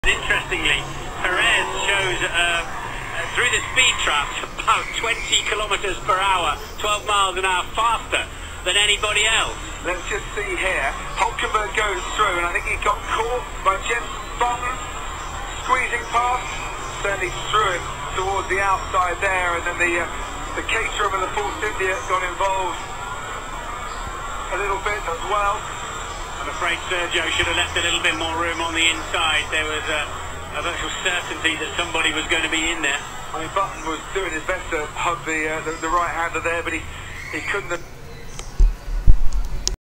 Checo Pérez choca en el GP de Mónaco 2014
Checo Pérez es tocado por Jenson Button en el inicio de la carrera de Mónaco 2014 que pena!!